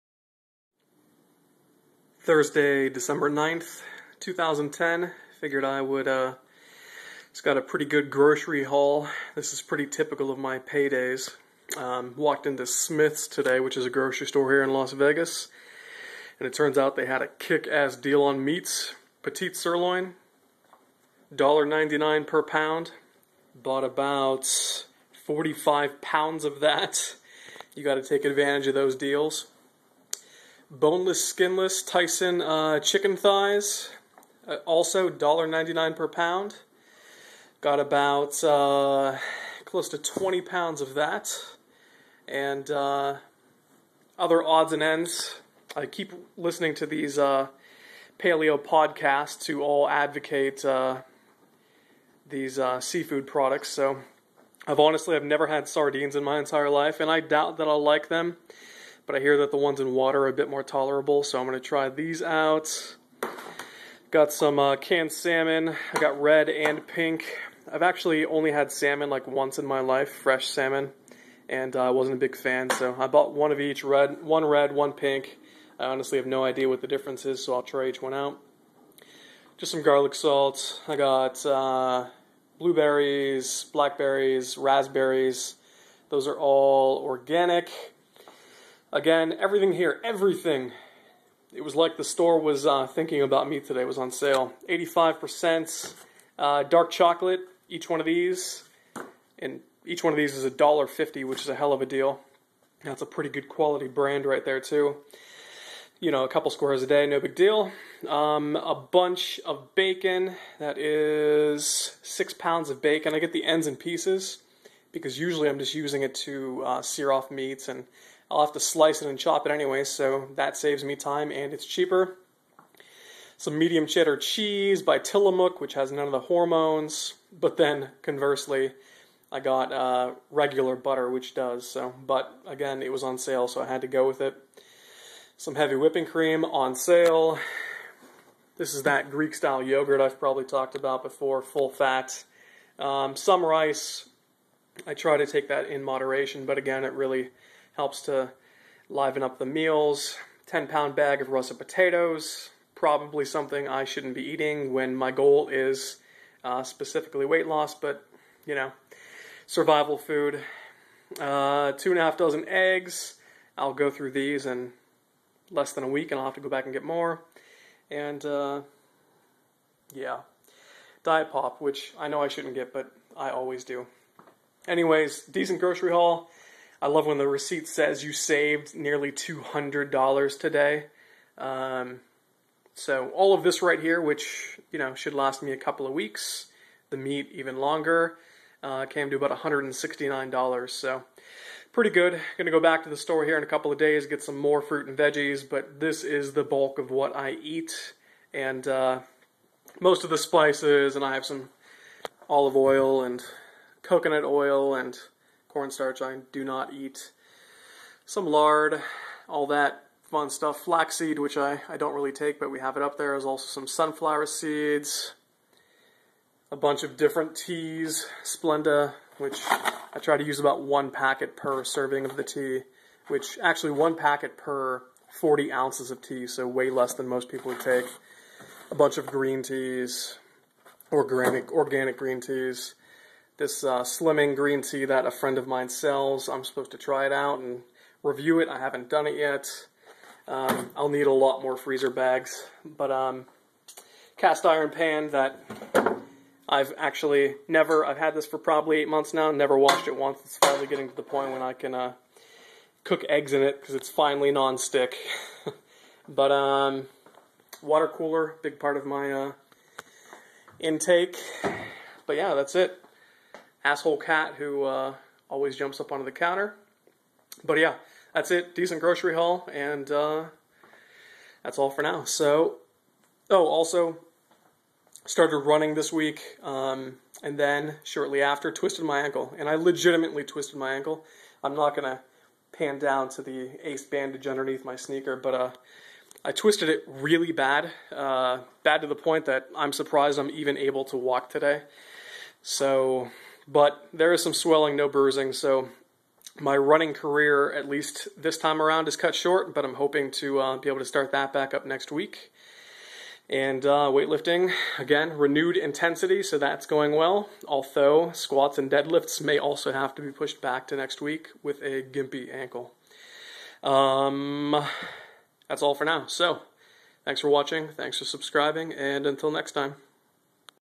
Grocery Haul: Vegans Beware!
A typical payday grocery haul. I didn't show it but there was a lot of frozen berries in the fridge and nuts/seeds in the cupboards. I know that I need to replace the starchy vege's with green ones but that's been a hard adjustment to make. I'm trying.